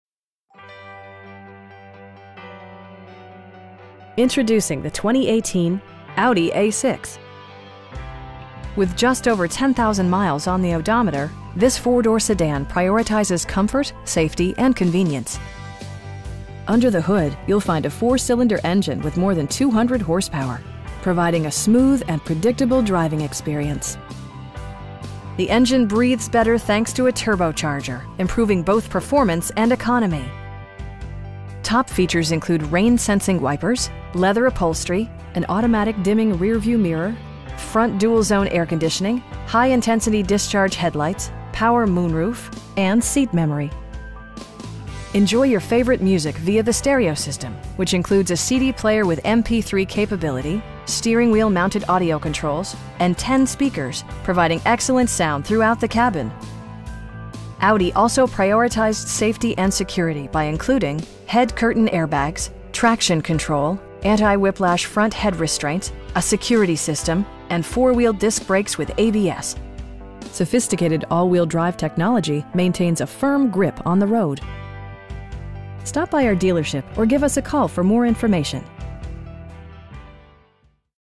2018 Audi A6 2.0T in Bridgewater, NJ 08807
Audi Bridgewater

Introducing the 2018 Audi A6. With just over 10,000 miles on the odometer, this 4 door sedan prioritizes comfort, safety and convenience. Under the hood you'll find a 4 cylinder engine with more than 200 horsepower, providing a smooth and predictable driving experience. Well tuned suspension and stability control deliver a spirited, yet composed, ride and drive The engine breathes better thanks to a turbocharger, improving both performance and economy. Top features include rain sensing wipers, leather upholstery, an automatic dimming rear-view mirror, front dual-zone air conditioning, high intensity discharge headlights, power moon roof, turn signal indicator mirrors, and seat memory. Audi also prioritized safety and security by including: head curtain airbags, front side impact airbags, traction control, brake assist, anti-whiplash front head restraints, a security system, and 4 wheel disc brakes with ABS. Sophisticated all wheel drive technology maintains a firm grip on the road. We pride ourselves in consistently exceeding our customer's expectations. Stop by our dealership or give us a call for more information.